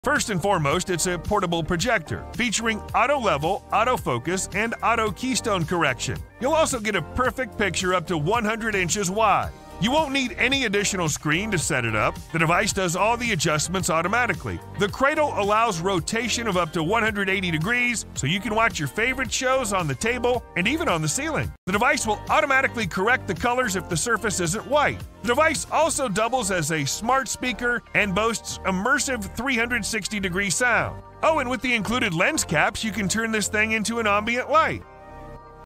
Samsung The Freestyle Projector ;Transform any space into a cinematic wonder
Say goodbye to traditional TVs and embrace a world of cinematic possibilities with the Samsung Freestyle Projector. This cutting-edge device will revolutionize the way you enjoy movies, TV shows, and more, in the comfort of your own space.
First and foremost it's a portable projector featuring auto-level autofocus and auto-keystone correction You'll also get a perfect picture up to 100 inches wide You won't need any additional screen to set it up The device does all the adjustments automatically The cradle allows rotation of up to 180 degrees so you can watch your favorite shows on the table and even on the ceiling The device will automatically correct the colors if the surface isn't white The device also doubles as a smart speaker and boasts immersive 360-degree sound Oh and with the included lens caps you can turn this thing into an ambient.
Don't miss out on this incredible device that's changing the way we experience entertainment.